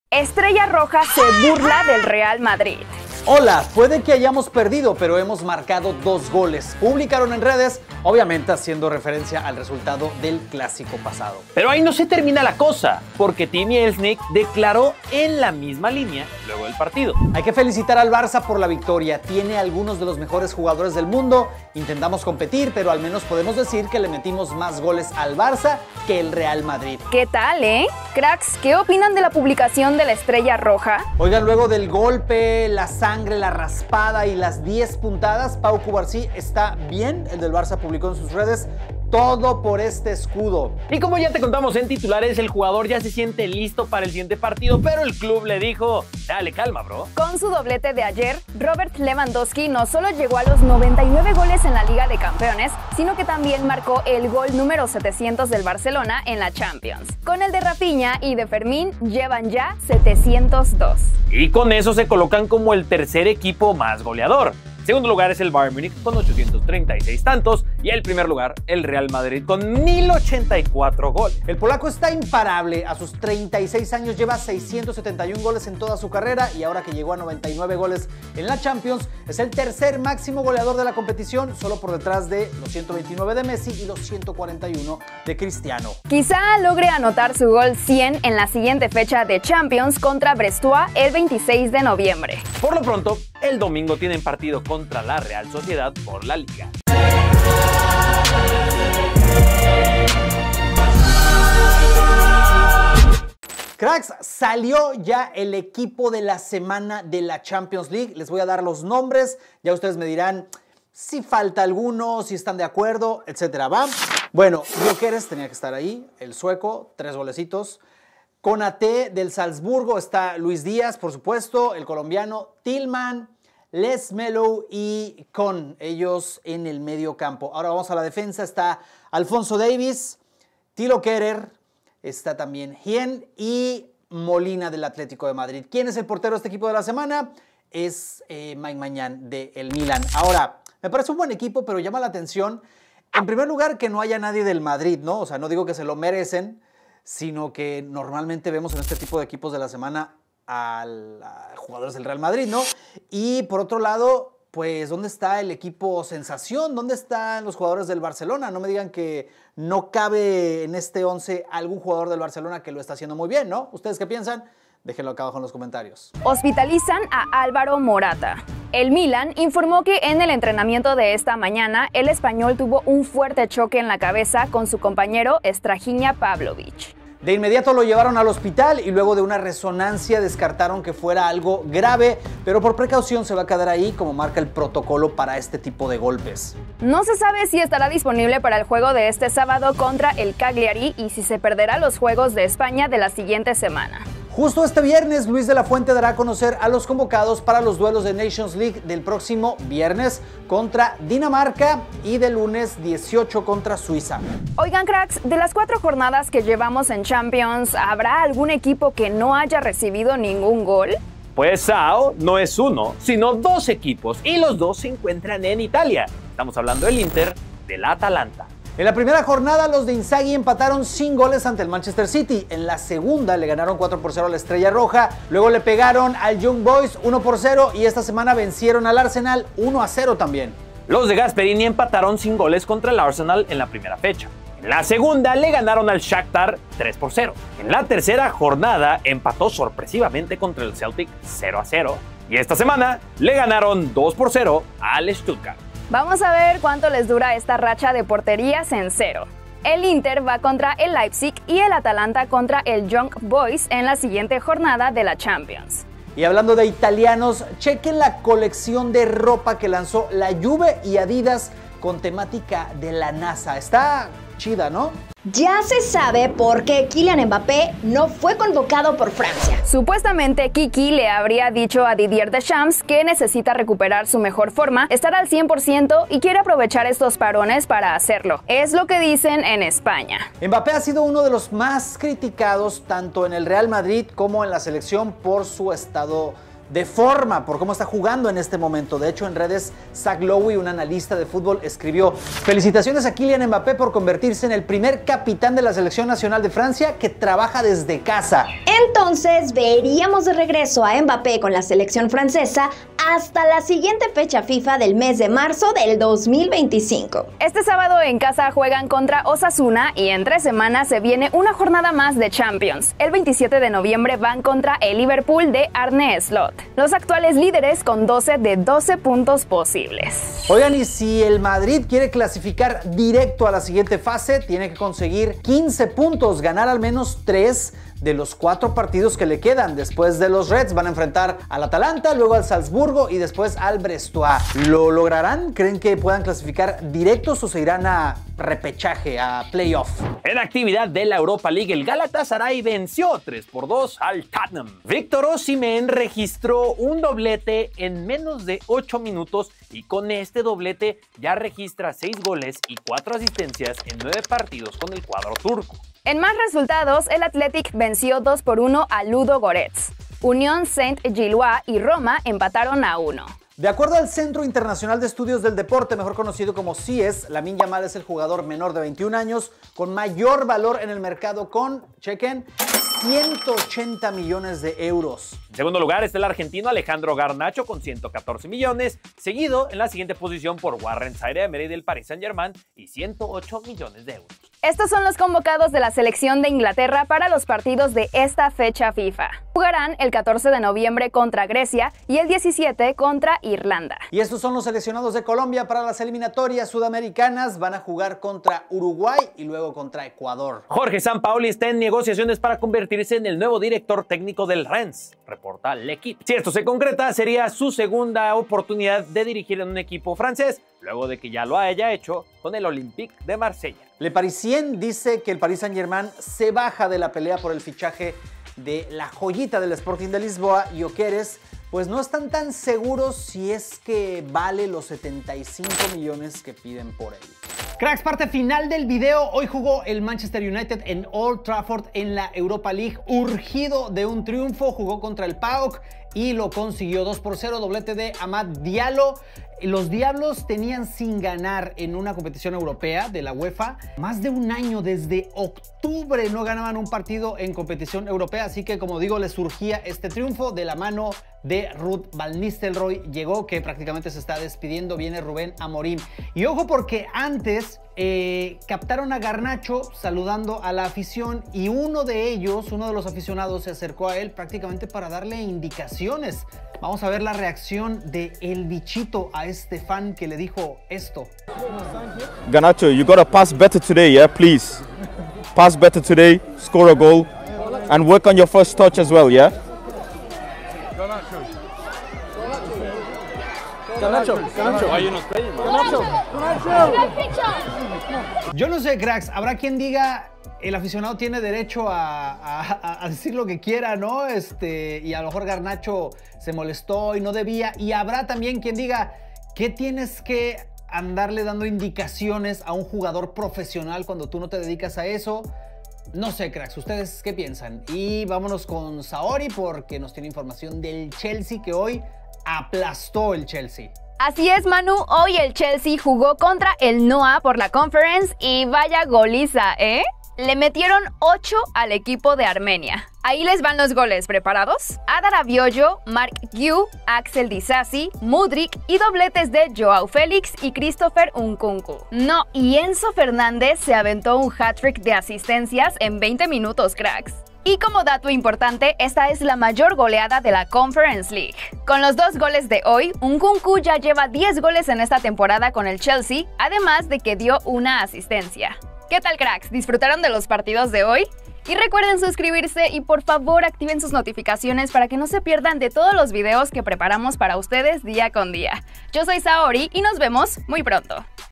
‘Trollearon’ al MADRID | Mbappe FUERA por esto | Arrasa Chelsea, United gana | Molestaron a Garnacho
Se burlan del Madrid. Cubarsí da todo por el Barca. Ya se seba por qué no convocaron a Mbappe. El Chelsea goleó. El united ganó. Un fan le exige a Garnacho y él responde así. Alvaro Morata al hospital. Robert Lewandowski imparable.